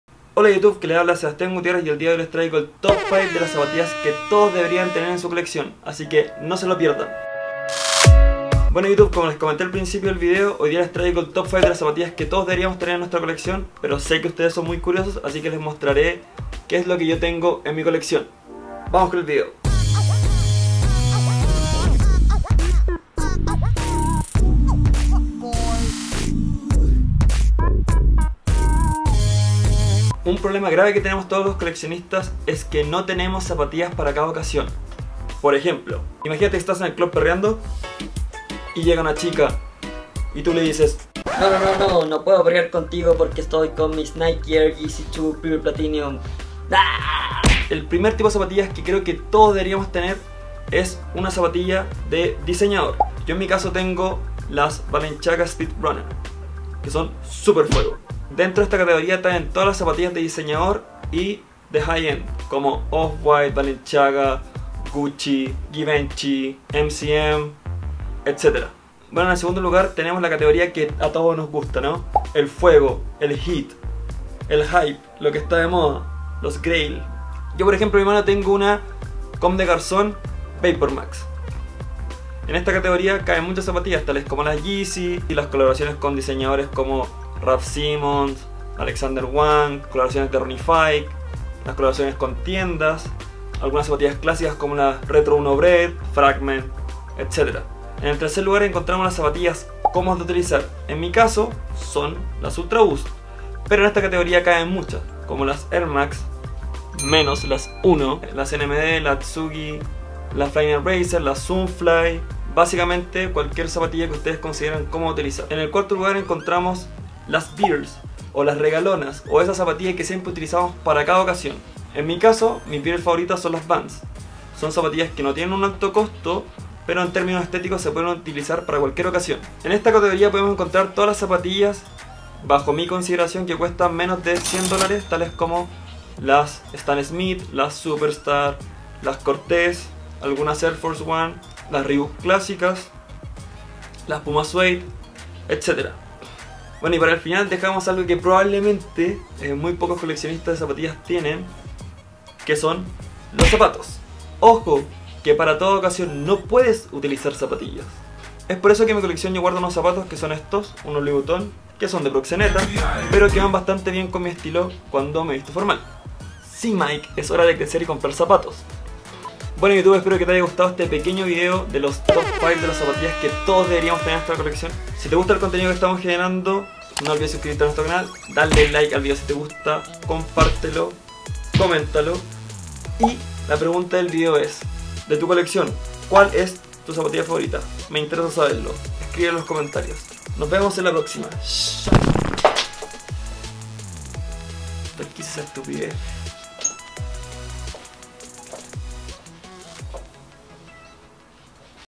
5 TIPOS DE ZAPATILLAS que deberías tener en tu colección | by.jetset
Hola Fam!, en este nuevo video les traigo los 5 tipos de zapatillas que todos debemos tener en nuestra colección. ¿ Cúal es su par favorito?.